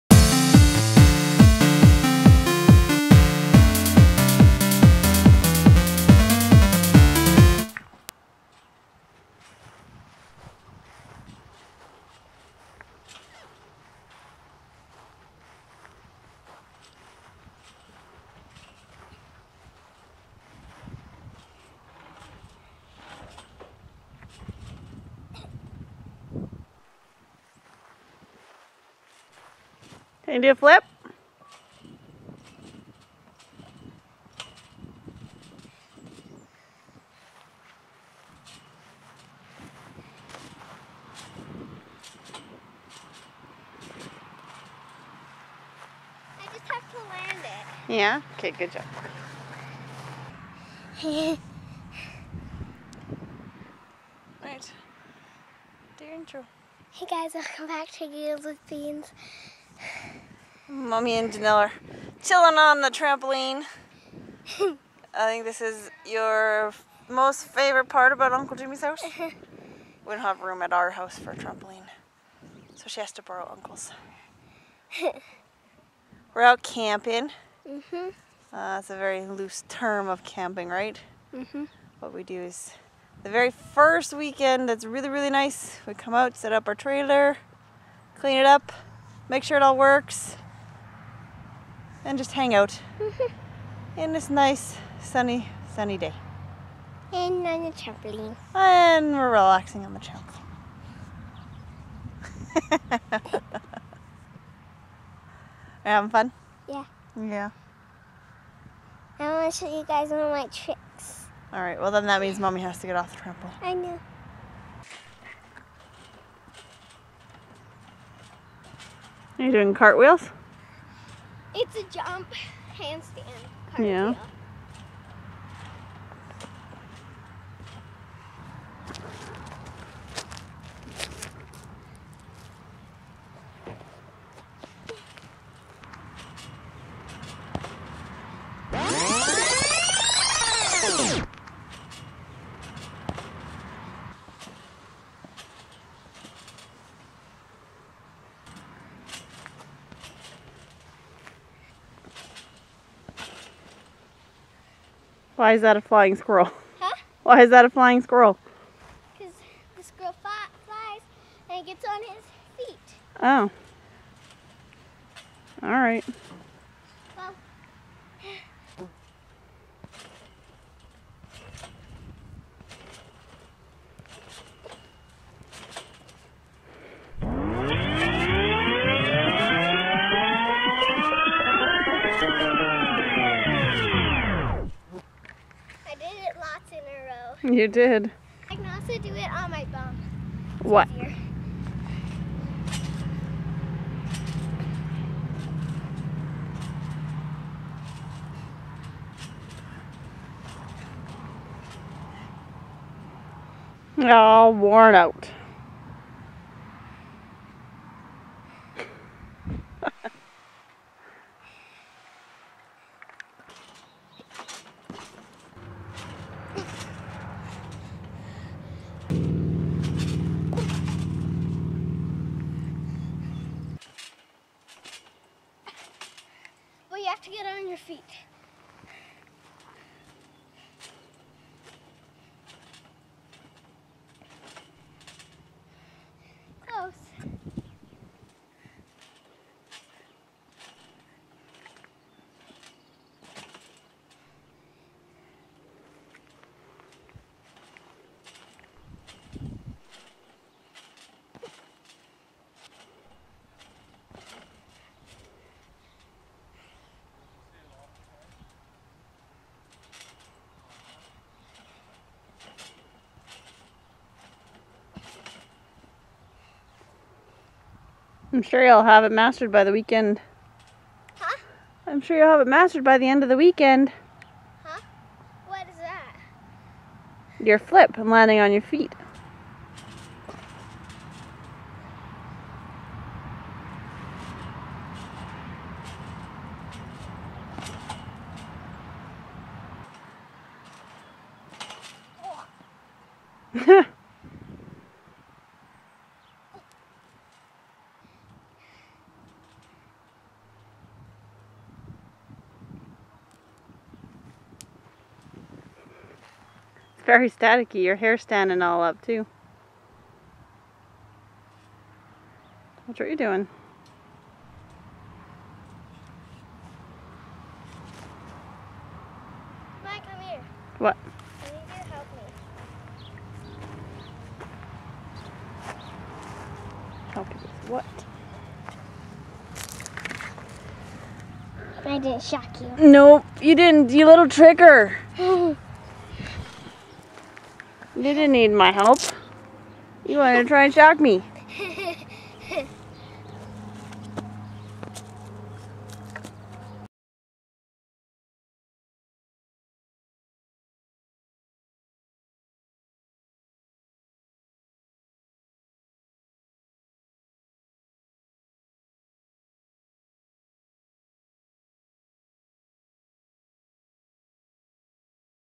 MAD TRAMPOLINE SKILLS
Miss beans shows off her trampoline gymnastics :D not bad considering she only gets to use the trampoline a few times per year and has never taken any classes or anything, she just has a lot of fun with it.

Beans loves receiving fan mail & we'll do our best to read all mail on the show
Send Mail to:
RPO ALBERT PARK
CALGARY AB T2A 4V3